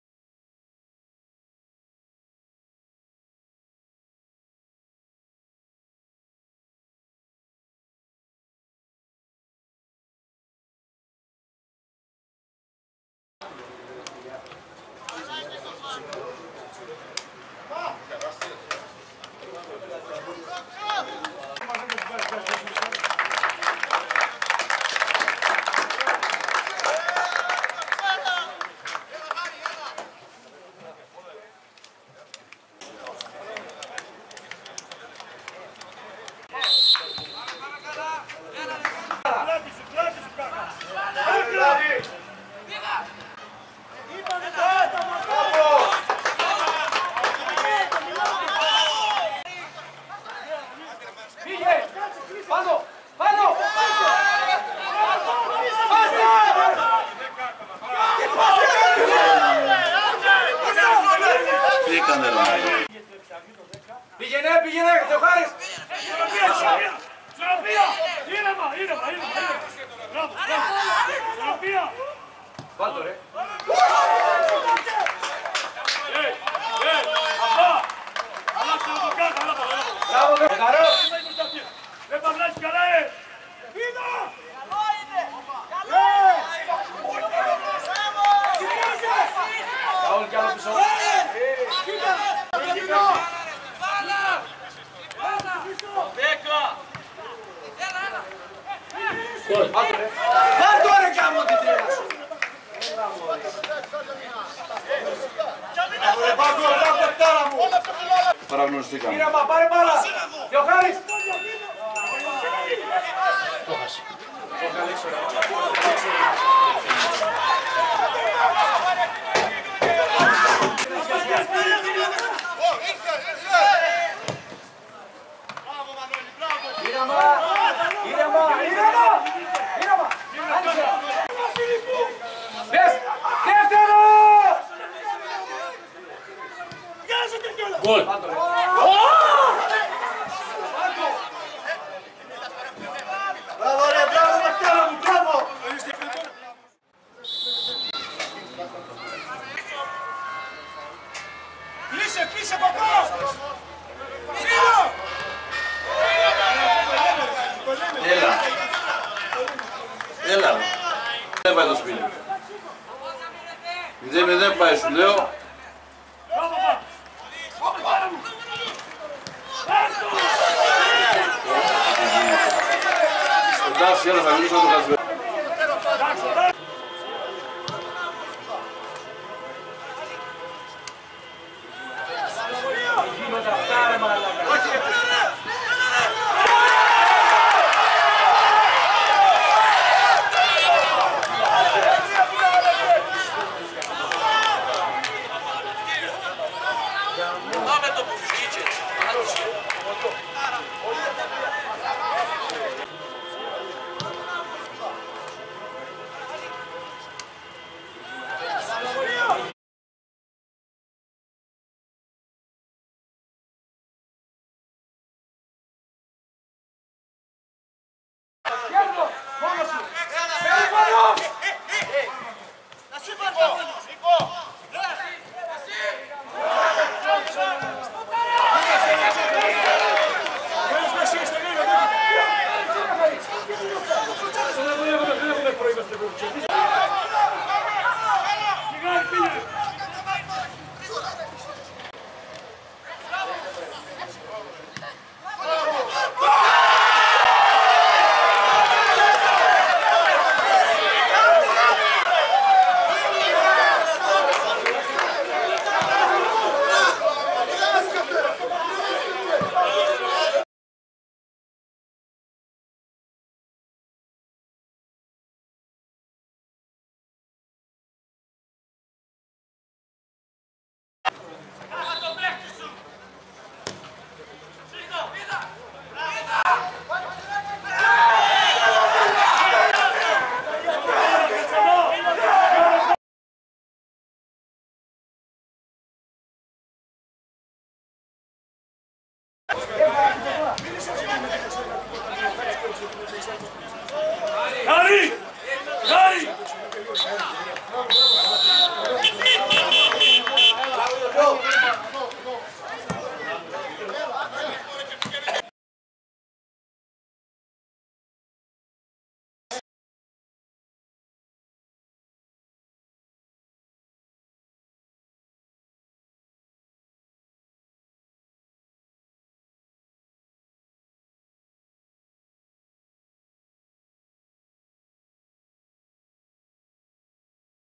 ΔΟΞΑ ΠΛΑΤΑΝΕ - ΚΑΛΥΒΟΣ 0-2 (best of) 6η ΑΓ. 23/10/16
ΔΟΞΑ ΠΛΑΤΑΝΕ - ΚΑΛΥΒΟΣ 0-2 Για την 6η αγωνιστική του Α' τοπικού Ρεθύμνου 23/10/2016
Οι καλύτερες φάσεις του αγώνα και τα γκολ! Video&Montaz by Yorgos Stefanakis